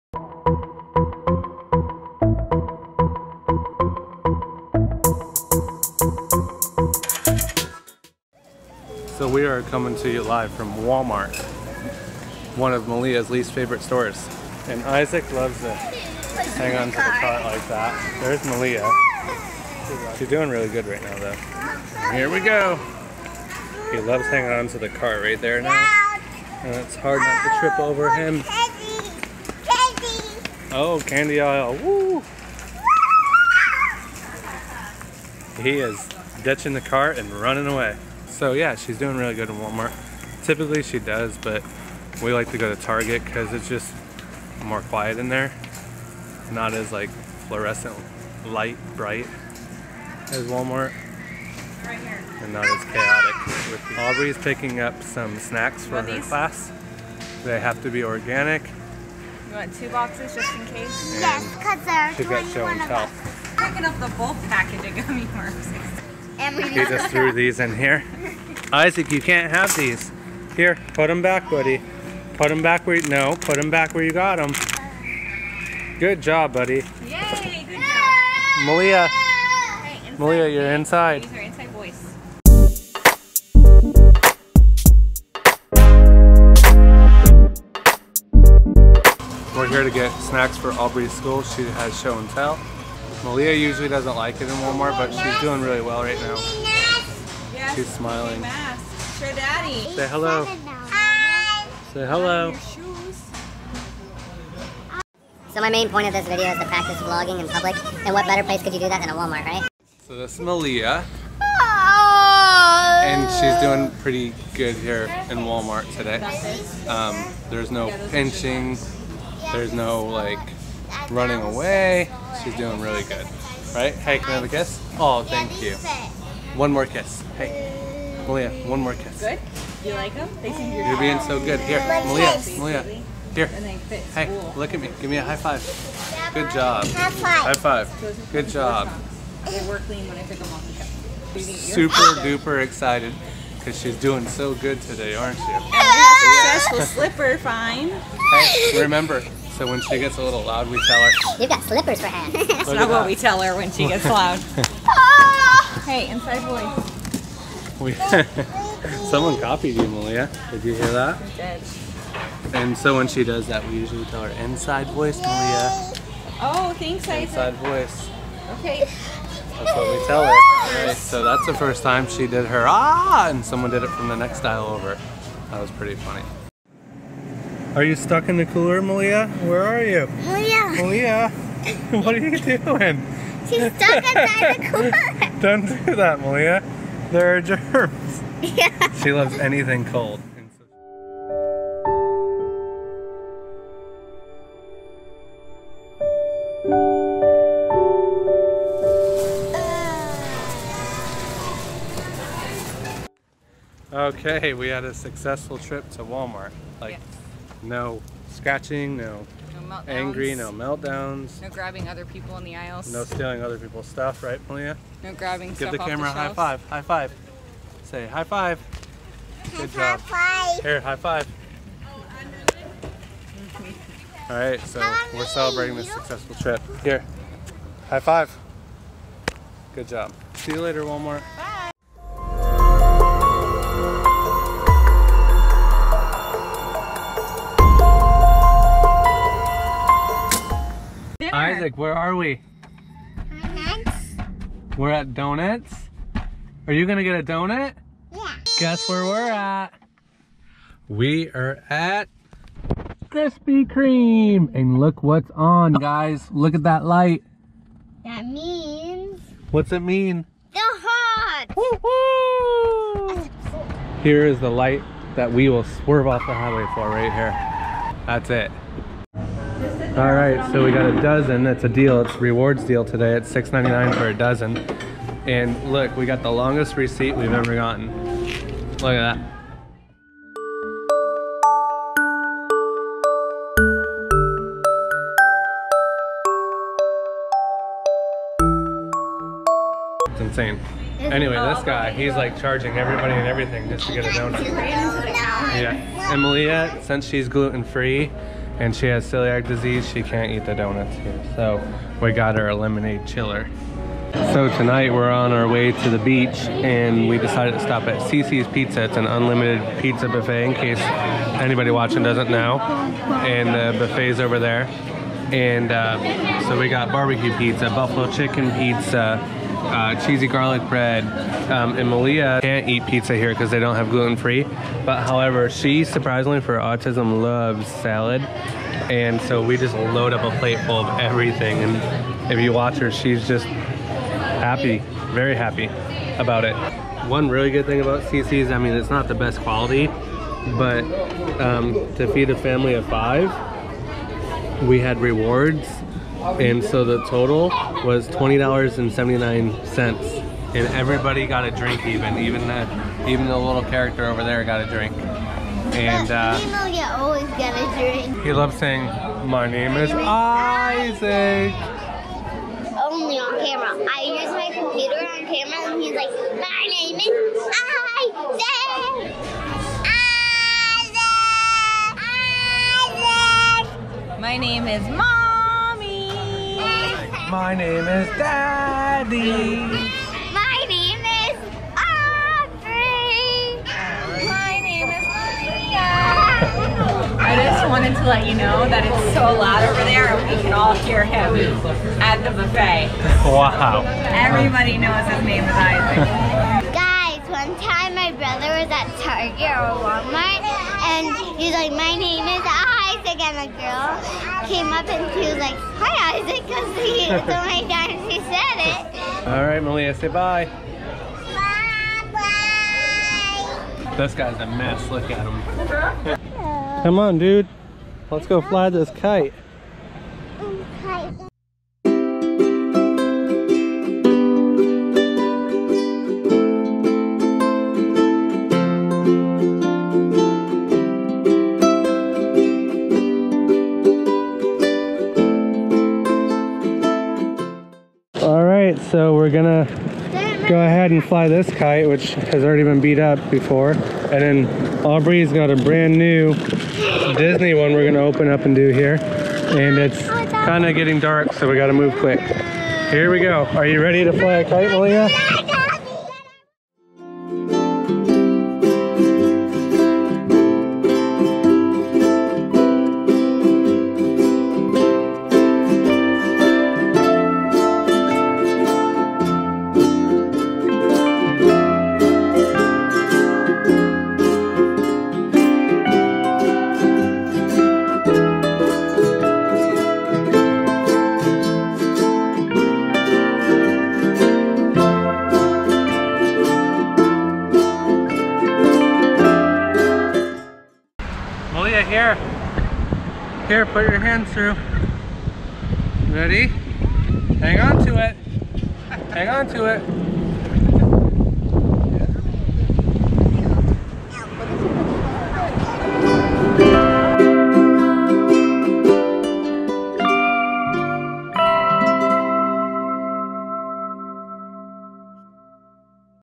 Autism ASD & Walmart, Krispy Kreme, CiCi's and Flying Kites!!!
autism spectrum disorder asd and Walmart usually don't go together.  Melia did really well this time and didn't experience any sensory overload.  We typically avoid Walmart and go to Target.  We have the Target Redcard which helps with the higher prices!  We then took a visit to our favorite donut shop Krispy Kreme.  The kids love it because you get to see how they make the donuts through a huge glass window! Melia cannot eat the donuts due to celiac disease but loves their slushies! Then off to dinner at an extremely affordable pizza buffet called CiCi's Pizza.  Once we were so full we couldn't walk we then headed to the beach to fly our kites!  It is starting to cool down a little bit in Florida!  They all had a great day including Melia!